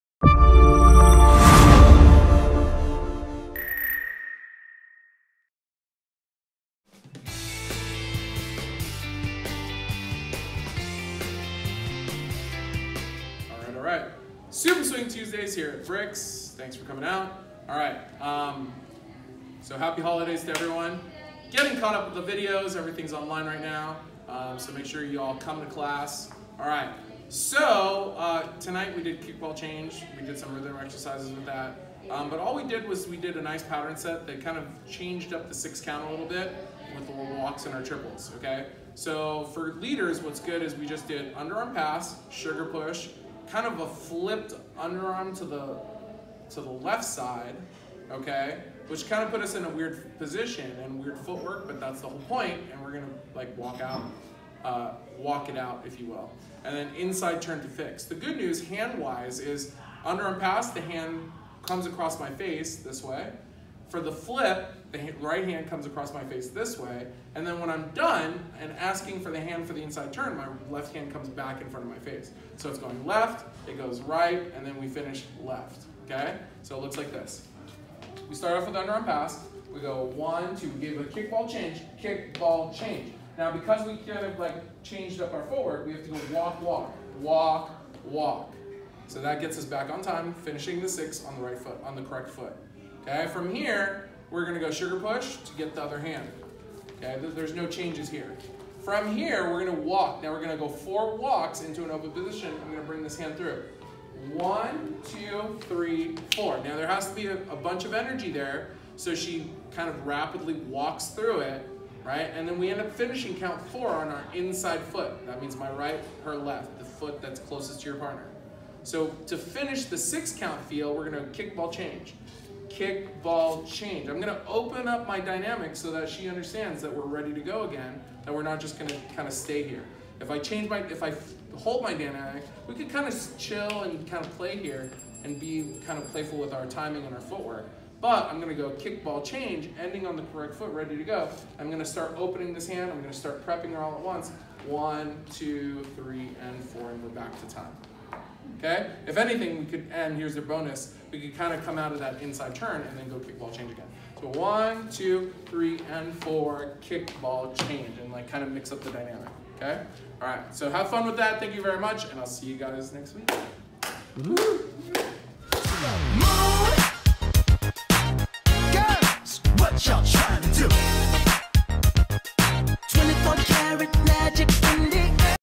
Super Swing Tuesdays Beg Class w/Arjay Centeno WCS @Brix December 5
December 5 West Coast Swing Class Video Recap

Drop in Classes every Tuesday in Belleville NJ at Brix

Because this is a drop in class, I teach this class to focus on movement and simple basic principles of partner dancing for West Coast Swing. More detailed instruction can be followed up with contacting the instructor for private instruction or doing follow up instruction with the instructor's DVDs available for purchase on arjaycenteno.com

Learning note*
In this class I constantly try to push the students in understanding a piece of my teaching philosophy within cool patterns and styling.

Keep in mind the dance of West Coast Swing is a versatile and ever changing dance, so the patterns, styling and teaching style in this video is my personal preference.